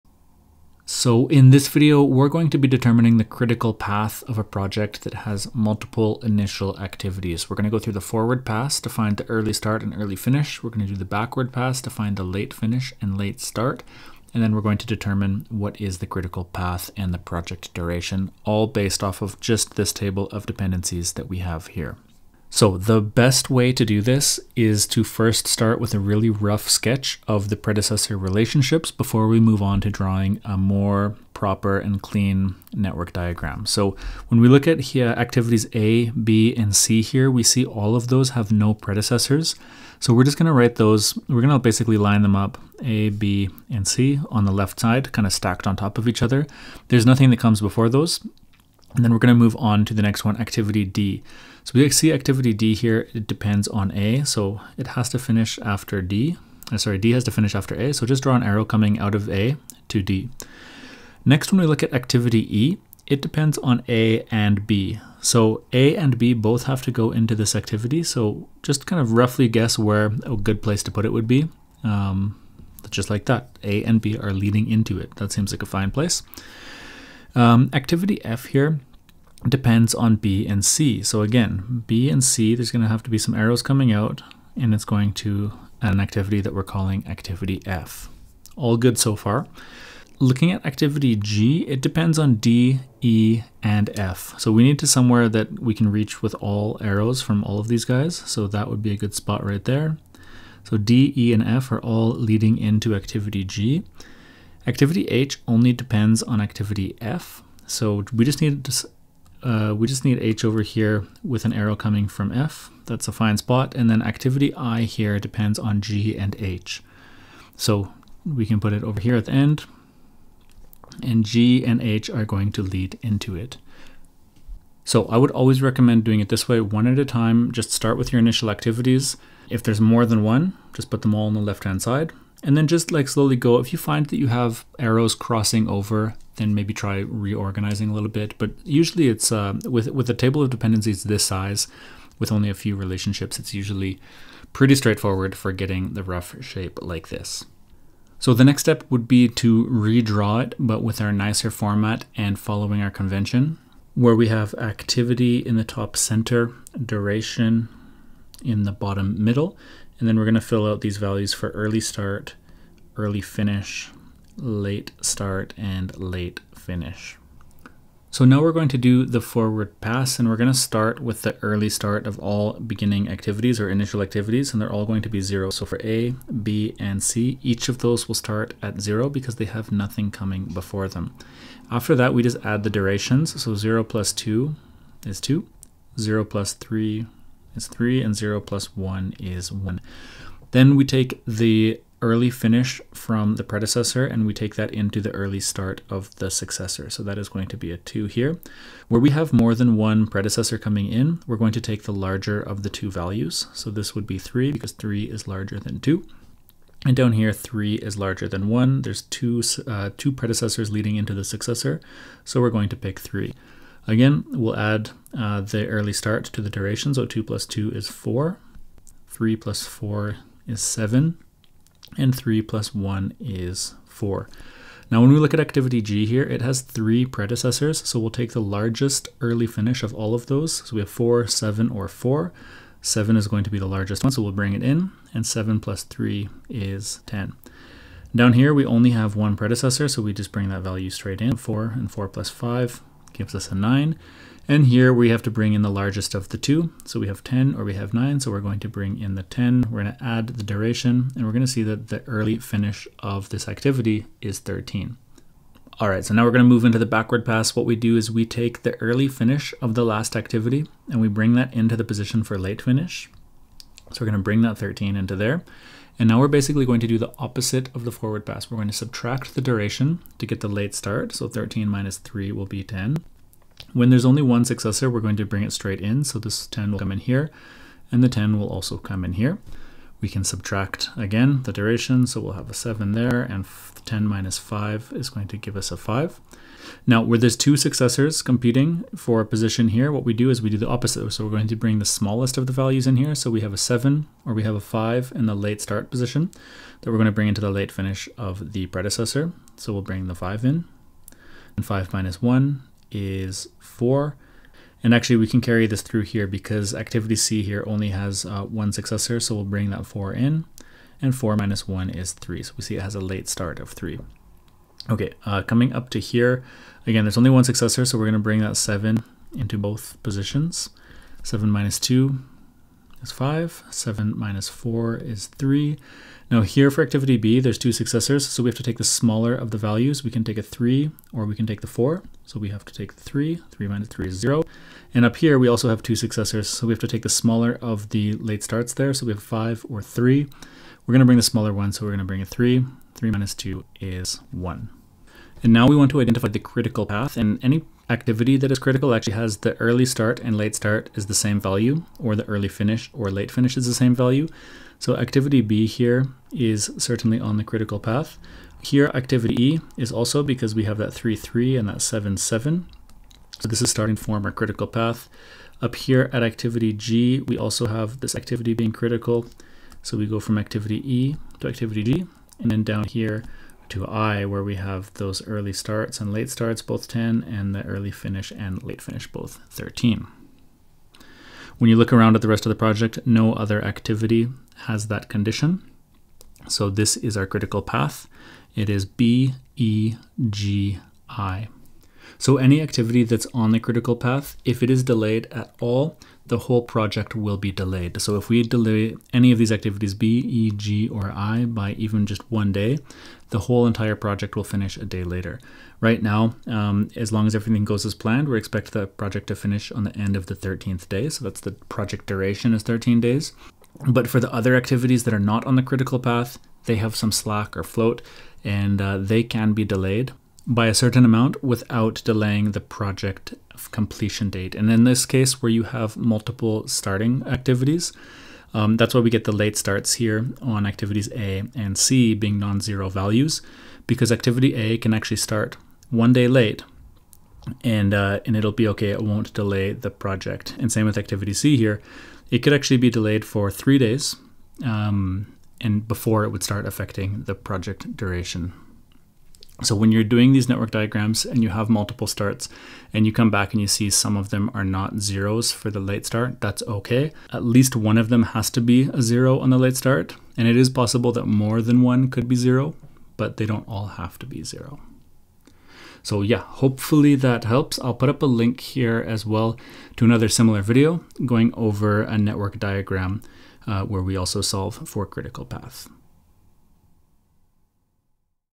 Critical Path of a Project with Multiple Initial Activities
This tutorial shows how to draw the network diagram and find the critical path of a project when there are multiple initial/starting activities. It is part of a full free course on project management.